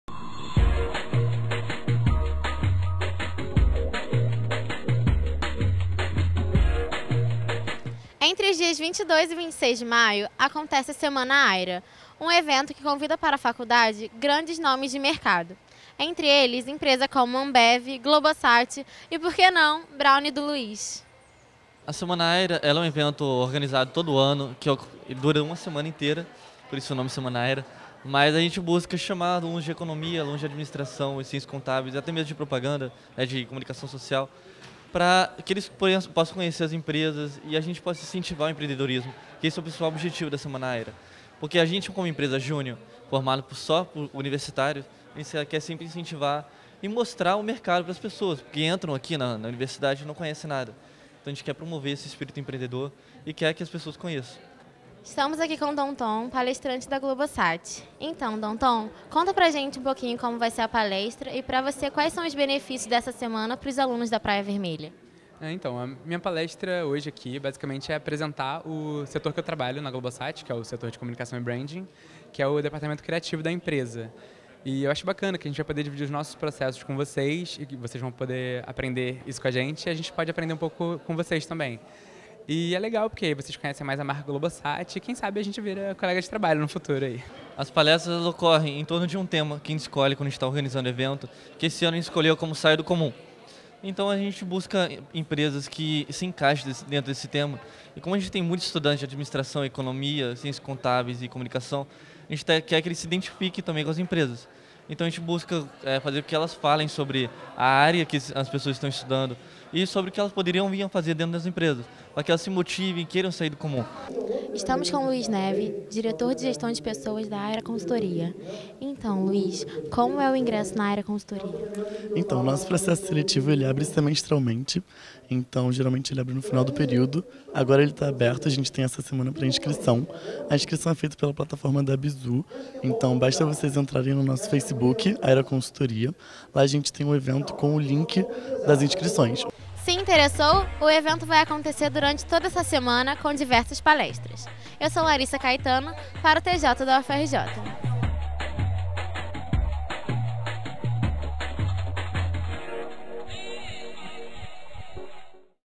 Semana Ayra 2017 na Praia Vermelha entre os dias 22 e 26 de maio
O evento da Semana Ayra promete inovar! Venha sair do comum e conhecer mais sobre o mundo do empreendedorismo e da Consultoria Ayra.

• EQUIPE
Repórter: Larissa Caetano
Câmera: Jaques Lucas
Produção: Igraínne Marques
Edição: Nathalia Couto e Igraínne Marques